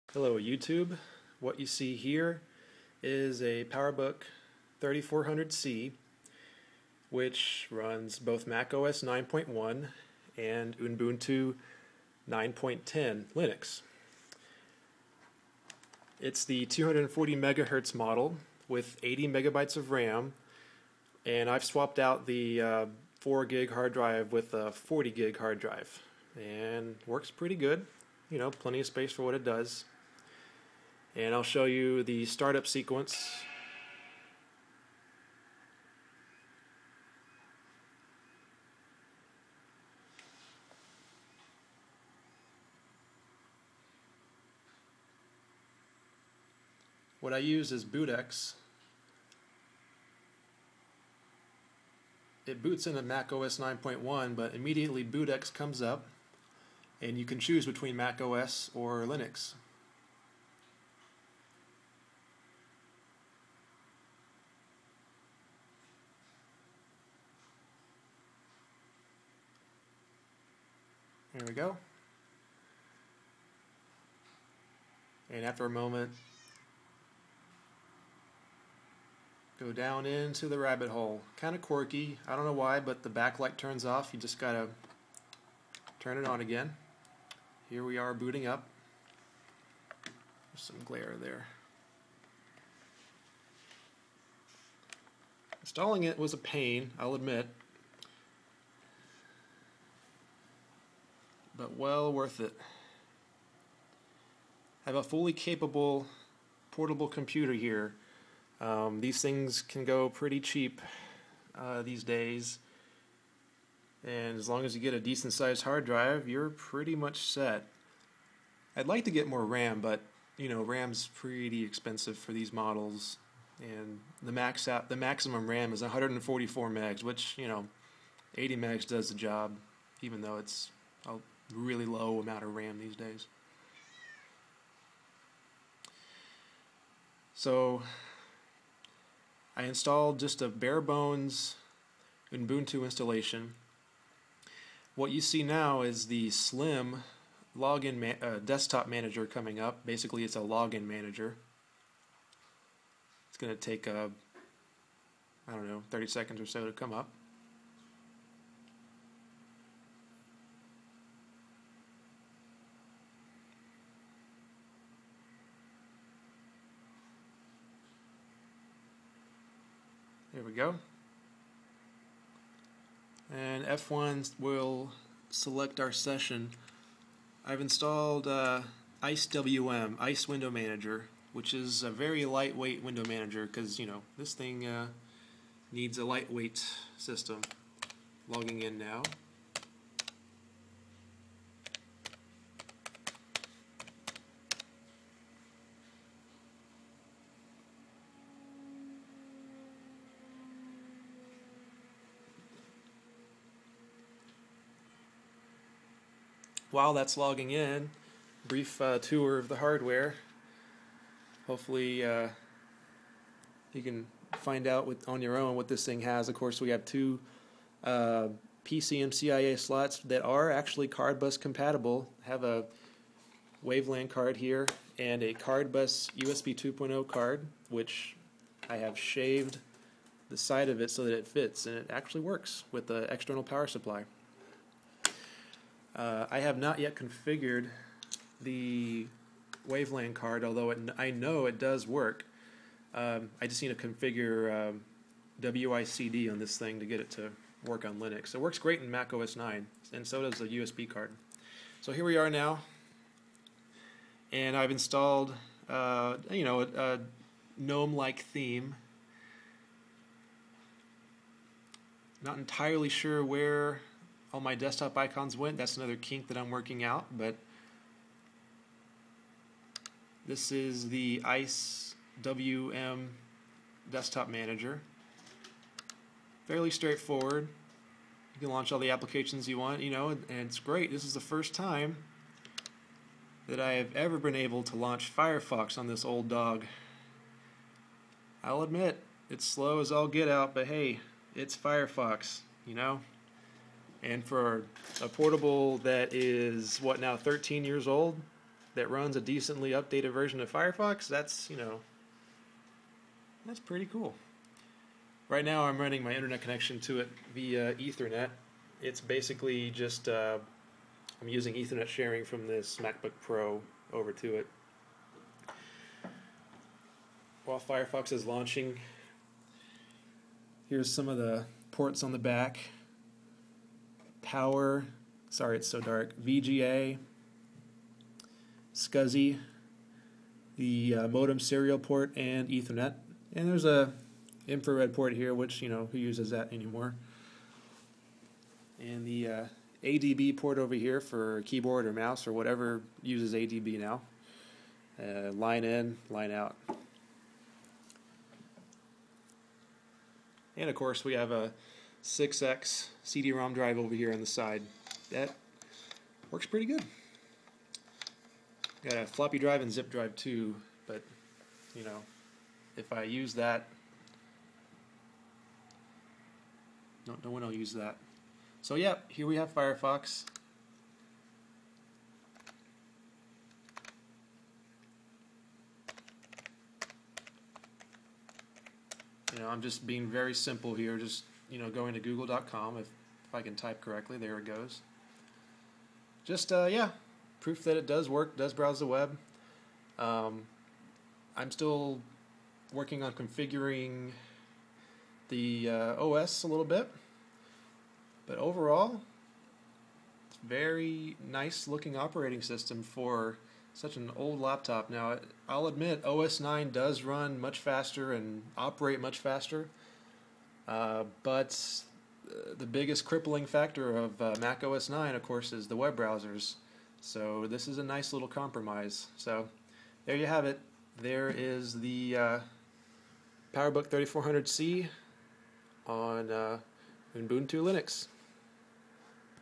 PowerBook 3400c running Ubuntu Linux 9.10
Before posting comments, please read this:
Fact: It's slower than Christmas. I know.
-- Think about its age, and specs. Then think about what it's successfully doing.

Fact: There are many browsers to choose from for the Linux platform, and FireFox is by far one of the most bloated and memory-hogging one of them all
-- and yet it runs on this system.
--  I've had success with other more lightweight browsers on Linux with this machine such as Gecko.

Fact: I could use Mac OS 8 or 9, and run Classilla.
-- I know Mac OS 9 can run Classilla, and that this is under active development. You don't need to post comments reminding me of that.

Here's a circa 1997 PowerBook 3400c running Mac OS 9.1, and also booting into Ubuntu 9.10 Linux.

It uses SLiM login manager, and Icewm GUI.

I installed Ubuntu using the Ubuntu PowerPC 9.10 Alternate Installer, and BootX. If desired, I can post more details about how I did it. It's not a process for the faint of heart. It would take several pages of text to fully explain how I did it. I can't point to one specific website of resources. I probably used the advice of 20 websites combined to complete the task, along with a great deal of experimentation on my own.

There's still a lot of configuration and work to do, but here's a brief video of startup sequence, and a tour of the hardware. Enjoy!

Specs:
Apple Macintosh PowerBook 3400c
Processor: PowerPC 603e 240 MHz
RAM: 80 MB
Hard disk: 40 GB 5400 RPM PATA
Display: 800 x 600 16-bit color
6X CD-ROM drive
Battery still holds a nice charge (an hour or more)
For specs of this computer when it was originally released,
5 GB partition with Mac OS 9.1
33 GB partition with Ubuntu 9.10